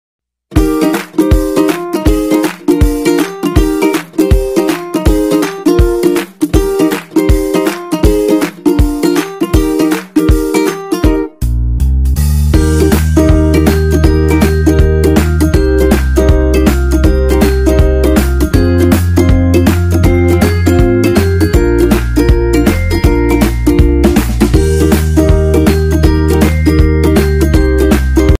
ART of ROBIN BIRD ALPHABET by KIDS CHANNEL 56
kids alphabet art
ART of ROBIN ALPHABET by LOCAL WORLD ART MAGIC
The purpose is to teach English alphabet and Vacabulary to kids.